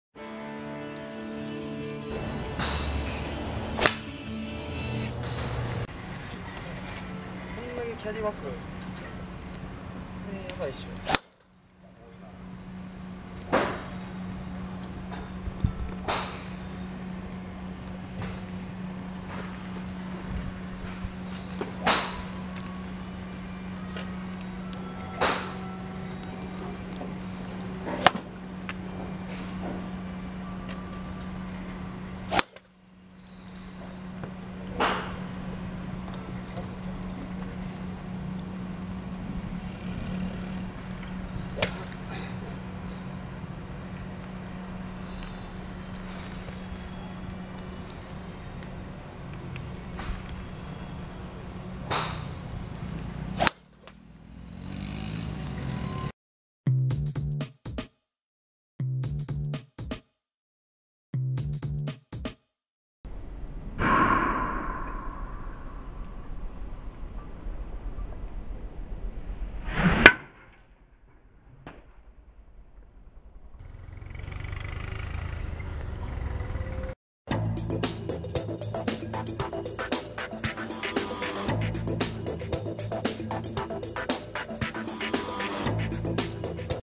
ゴルフWGSL練習風景Toshi編vol.36捩じれのないストレートドロードライバーショット！【Toshi】WGSLスイングコンサルレッスンgolfドライバードラコンアイアンアプローチパター
WGSL動画出演者の中で最も人気の高いスイングをする１人、Toshi君のドライバーショット動画です。

今回の動画も、彼のスイングアクションを見るだけで前倒しが分かるし、ひっくり返すというアクションが理解出来ると思います。

※WGSLチャンネル動画は、レッスン動画チャンネルではありません※

●頂戴するコメント対応について･･･の記事
WGSLへのコンタクトはE-mailからのみです

』でWGSLのジュニア・学生たちの応援ヨロシクお願いします！